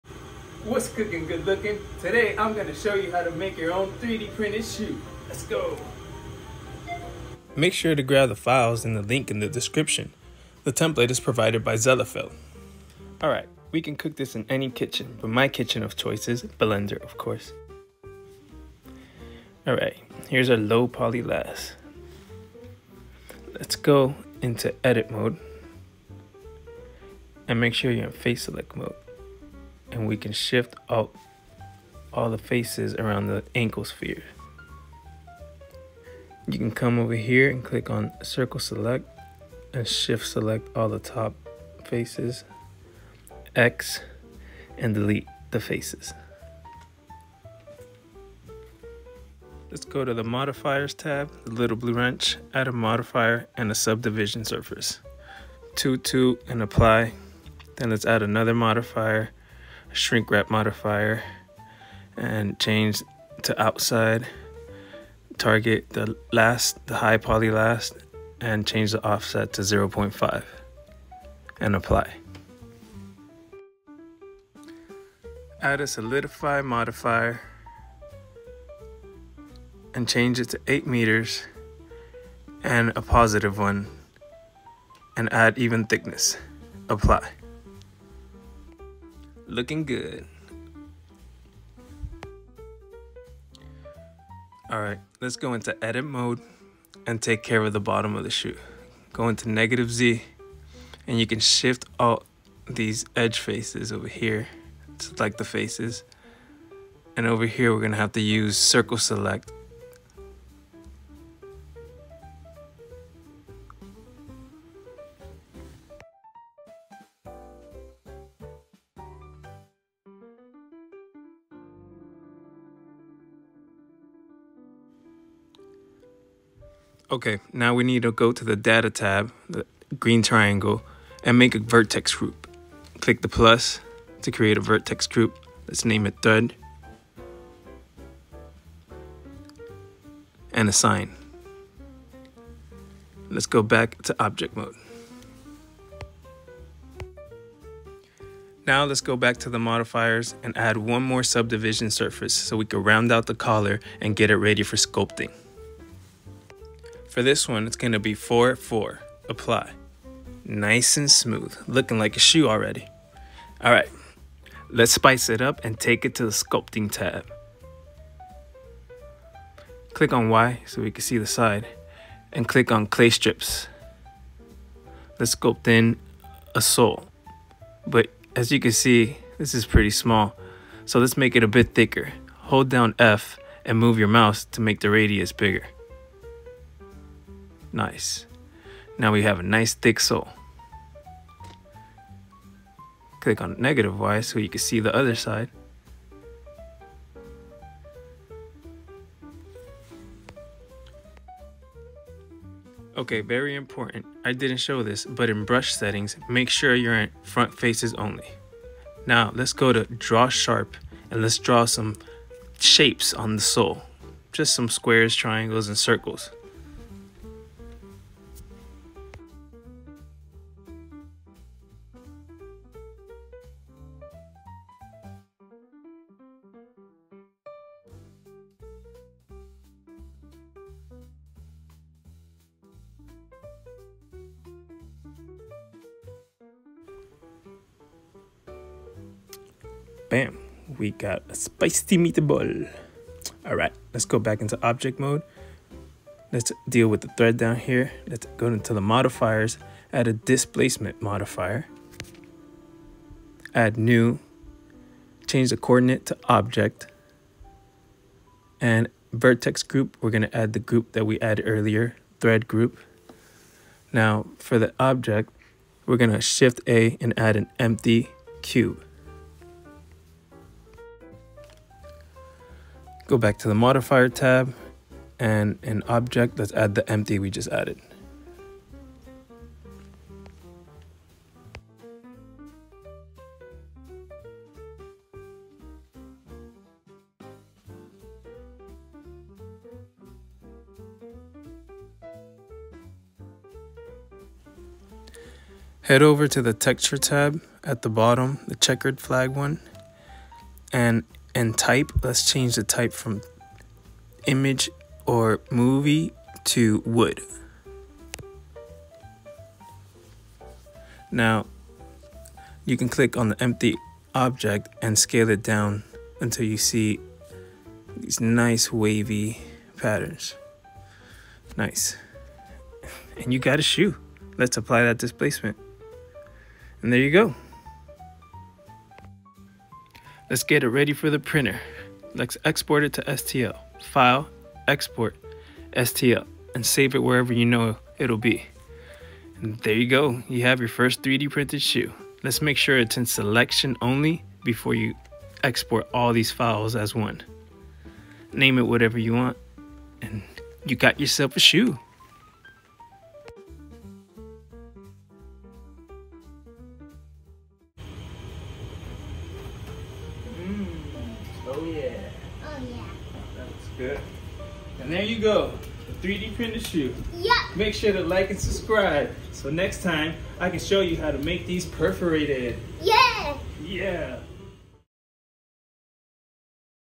The Secrete Recipe for a 3D-Printed Shoe! (Blender Tutorial)
🔥 Cook Up Your Own 3D-Printed Shoe with The Print Chef! 🍳

Ready to whip up some custom footwear? In this step-by-step tutorial, I’ll show you how to turn Zellerfeld’s low-poly last template into a sleek, print-ready shoe—all while keeping it smooth, spicy, and fun. Whether you’re a Blender wizard or just stepping into 3D design, this recipe is for you!

👟 What’s in the Recipe?
1️⃣ Trimming the low-poly last for a perfect fit
2️⃣ Adding a Subdivision modifier to smooth it out
3️⃣ Using Shrinkwrap magic to snap it to the high-poly version
4️⃣ Solidifying the shoe for that durable finish
5️⃣ Sculpting custom tread designs
6️⃣ Adding a wavy sole texture with the Displace modifier
7️⃣ Exporting your masterpiece as an STL

Let’s get printing and stay spicy! 🌶️